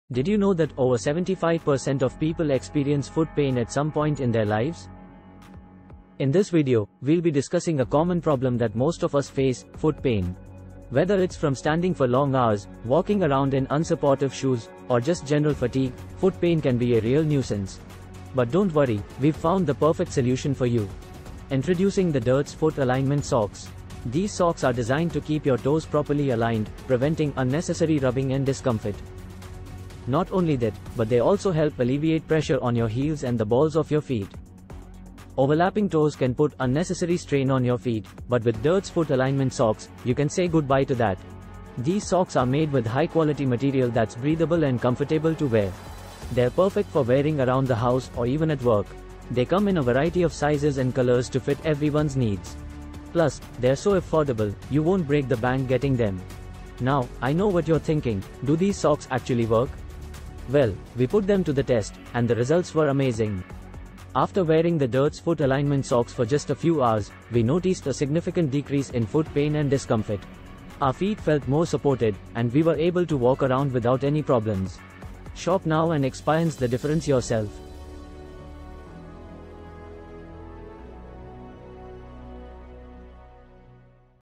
Step Up Your Foot Care Game with DIRTS Alignment Socks
Introduction:
Welcome to our video on DIRTS Foot Alignment Socks! In this video, we will show you how these socks can help you get rid of foot pain and correct your posture, so you can walk, run, and stand with confidence.

Video Content:

Are you experiencing foot pain or discomfort? Do you have trouble standing or walking for long periods of time? If so, DIRTS Foot Alignment Socks may be the solution you've been looking for.

These socks are specially designed to improve your foot alignment and provide support where you need it most. They have a unique arch support system that helps to distribute weight evenly across your feet, reducing pressure and pain.

DIRTS Foot Alignment Socks also feature a toe separator that helps to realign your toes and improve your overall posture. This can have a positive impact on your entire body, reducing pain and improving your overall comfort level.

In addition to their functional benefits, these socks are also stylish and comfortable. They are made from high-quality materials that are soft, breathable, and moisture-wicking, so your feet stay cool and dry all day long.

Whether you're an athlete looking for performance-enhancing gear or someone who simply wants to improve their foot health, DIRTS Foot Alignment Socks are a must-have.

Social Media Links:

for more tips and tricks on foot health and wellness.